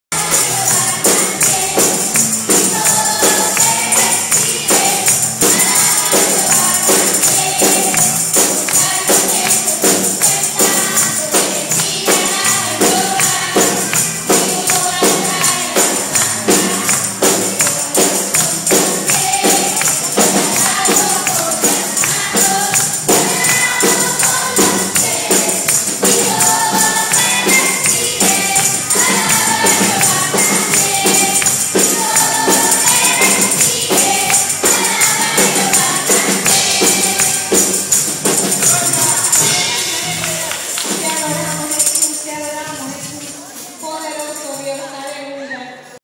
Coro Pentecostal de Niños II
Culto Evangelistico, 27 de febrero de 2012, IDDPMI. San Sebastian, Pueblo.